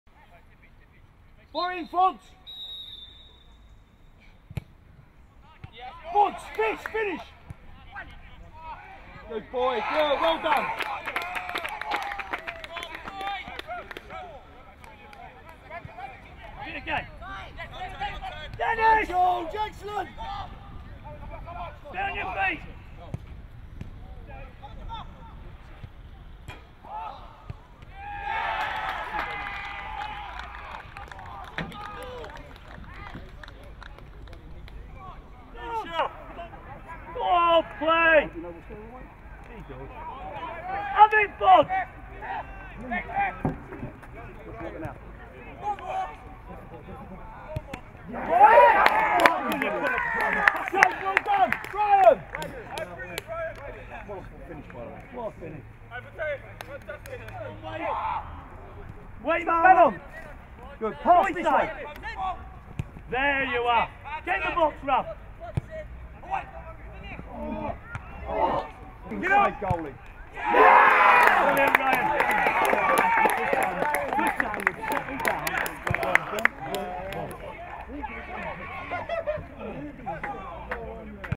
U18s: Fulham 3-1 West Ham United
Watch the goals as Aron Davies, Josh Walker and Ryan Sessegnon were all on target in a 3-1 home win against the Hammers.